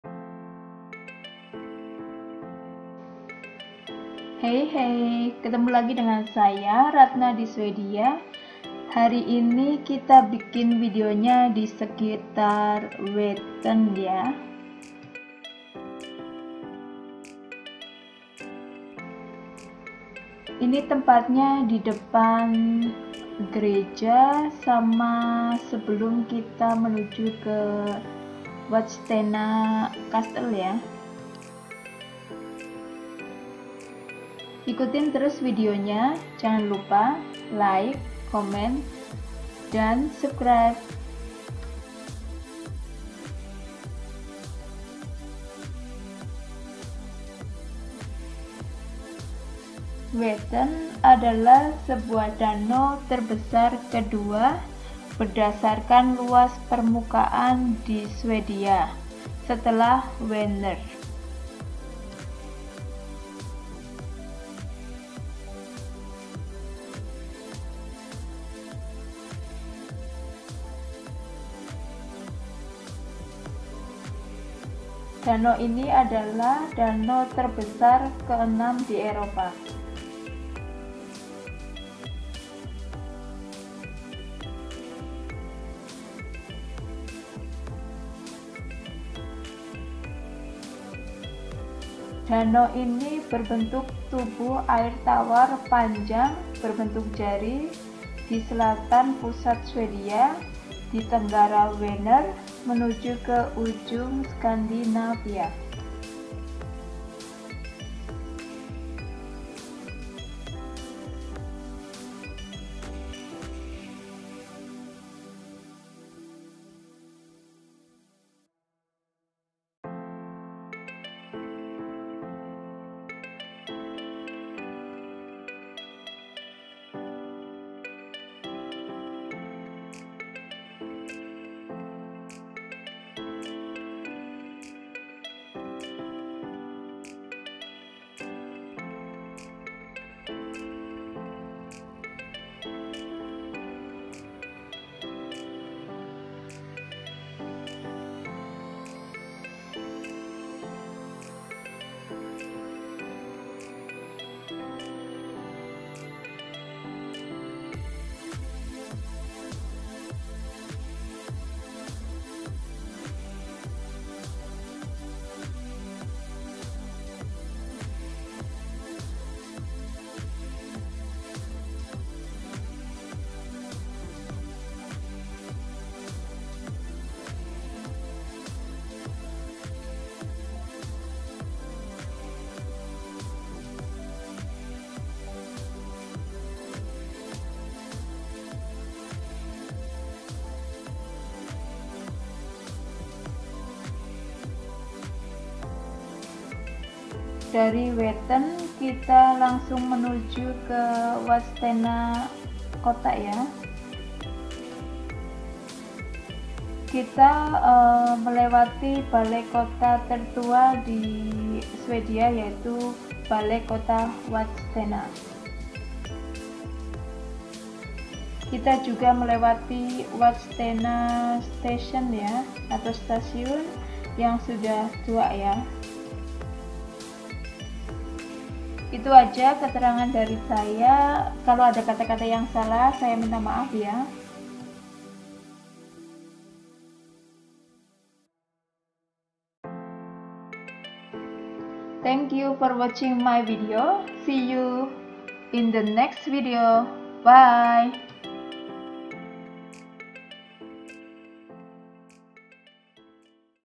Vättern 🇸🇪 (Danau terbesar kedua di Swedia) and Vadstena. 20200614
One of the etymologies for the name Vättern is from "vatten", the Swedish word for water. Vättern is the second largest lake by surface area in Sweden, after Vänern, and the sixth largest lake in Europe.
It is a long, finger-shaped body of fresh water in south central Sweden, to the southeast of Vänern, pointing at the tip of Scandinavia.
The lake's total surface area is about 1,912 km2 (738 sq mi), with a drainage basin a little over double that, about 4,503 km2 (1,739 sq mi).
The deepest known point, located to the south of the island of Visingsö, is 128 meters (420 ft).
The average depth is 41 meters (135 ft). The lake has a perimeter of about 642 km (399 mi). The volume is 77.0 km3 (18.5 cu mi). These numbers tend to be fixed, as the level of the lake is regulated.

Situated in Götaland, the lake is drained by Motala ström, starting at Motala, and flowing ultimately through a controlled canal into the Baltic Sea. The lake includes the scenic island of Visingsö, located outside Gränna. Other towns on the lake include Vadstena, Jönköping, Hjo, Askersund, Åmmeberg and Karlsborg. It is bounded by the Provinces of Västergötland, Närke, Östergötland and Småland.

In the north there is a scenic but not mountainous inland fjord, Alsen. About 62% of the drainage basin is still covered with spruce, pine and deciduous forest. About 26.7% is dedicated to agriculture.